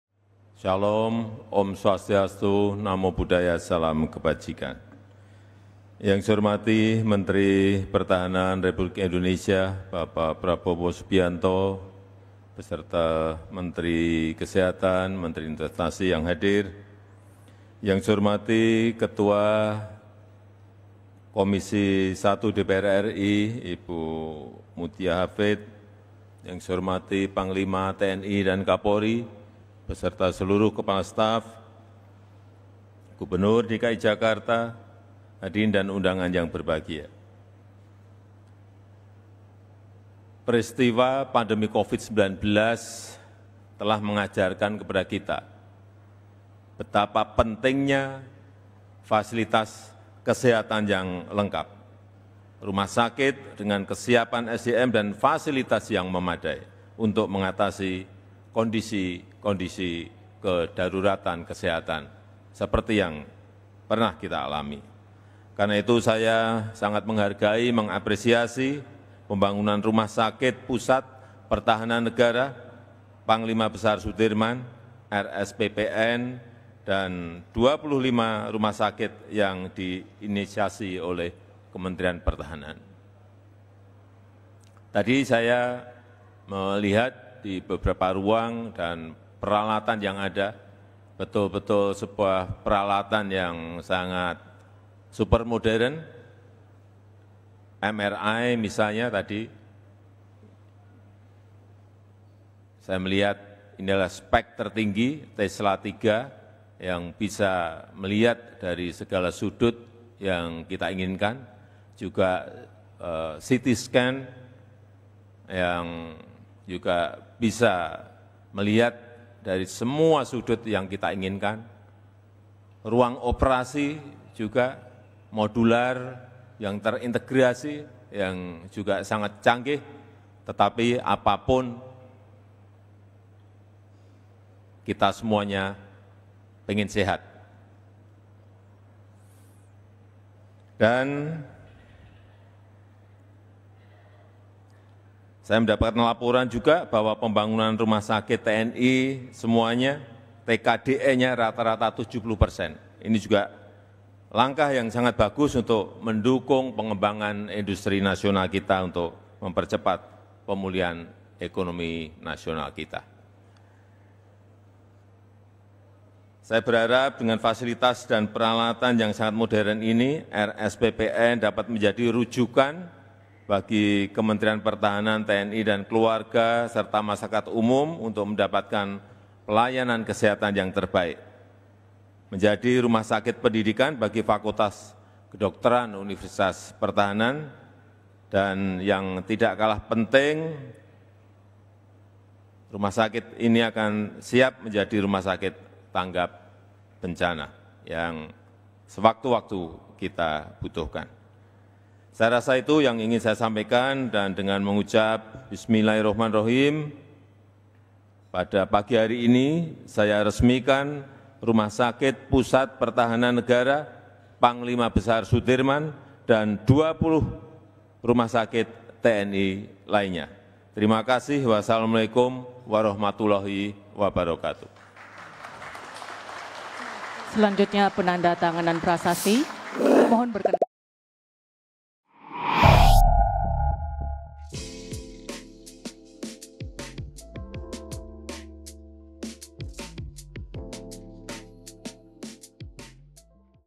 [FULL] Momen Puja Puji Presiden Jokowi, Hasil Kerja Menhan Prabowo Subianto ini
MERDEKA.COM - Menhan Prabowo Subianto meresmikan Rumah Sakit Pusat Pertahanan Negara Pangsar Soedirman dan 25 Rumah Sakit TNI, pada Senin 19 Februari 2024.

Presiden Jokowi menekankan, pentingnya infrastruktur kesehatan yang kuat berkaca dari pandemi Covid-19. Jokowi mengapresiasi pembangunan 26 rumah sakit TNI di seluruh penjuru negeri.